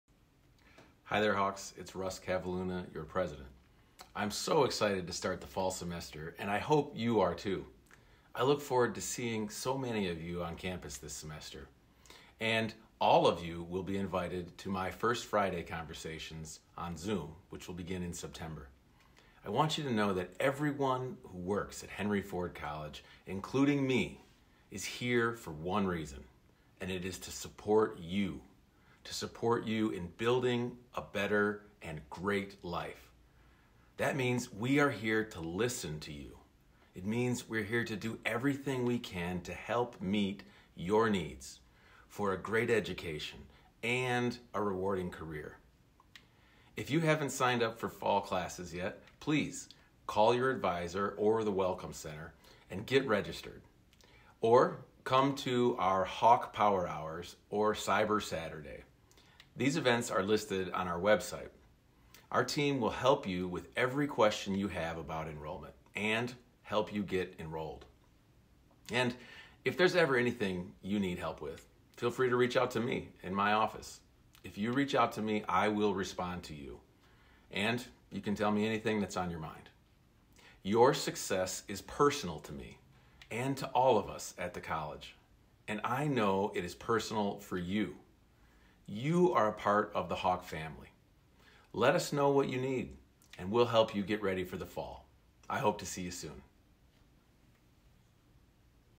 Welcome back, Hawks! How can we help you prepare for Fall 2022 semester?
President Kavalhuna welcomes students back for Fall semester, which starts August 25, and offers help with whatever needs you may have to get ready for Fall.